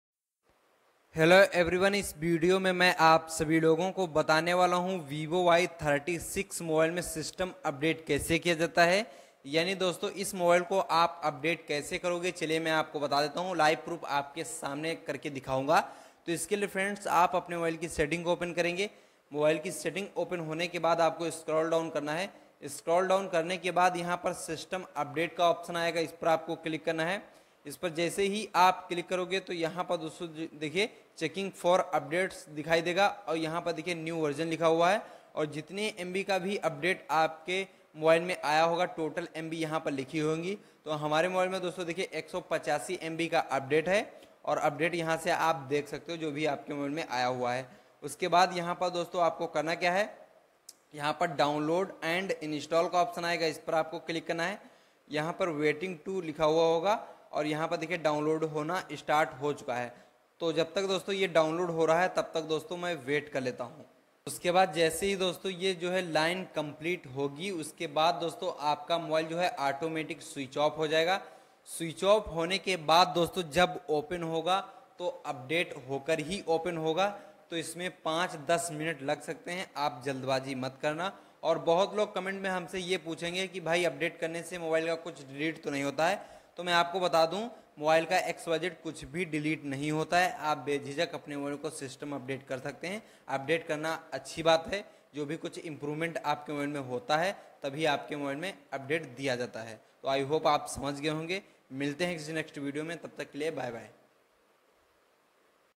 vivo y36 me system update kaise kare | how to system update in Vivo y36 | software update
hello doston is video mein Maine aapse bhi logon ko bataya hai Vivo y36 mobile mein system update kaise kiya jata hai